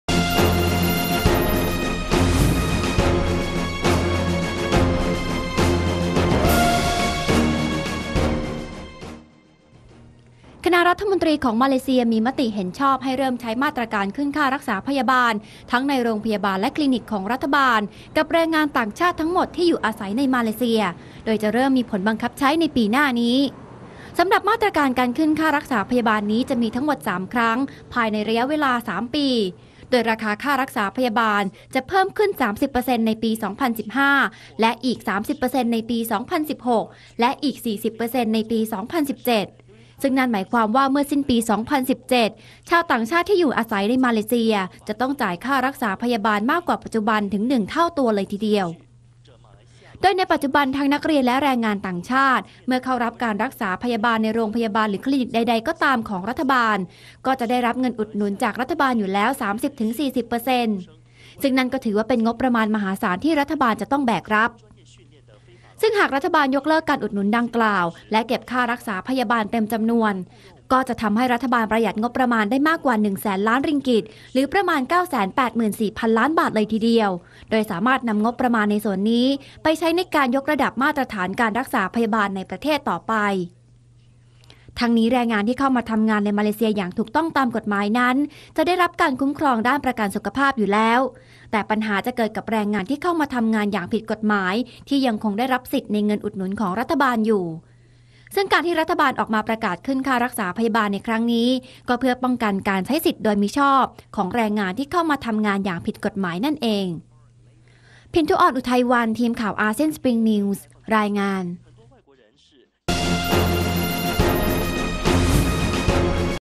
ครม.มาเลเซียมีมติขึ้นค่ารักษาพยาบาลแรงงานต่างชาติ - Springnews
คณะรัฐมนตรีของมาเลเซียมีมติเห็นชอบให้เริ่มใช้มาตรการขึ้นค่ารักษาพยาบาลทั้งในโรงพยาบาลและคลีนิคของรัฐ กับแรงงานต่างชาติทั้งหมดที่อยู่อาศัยในมาเลเซีย โดยจะเริ่มมีผลบังคับใช้ในปีหน้านี้ติดตามได้ใน อาเซียนอัพเดท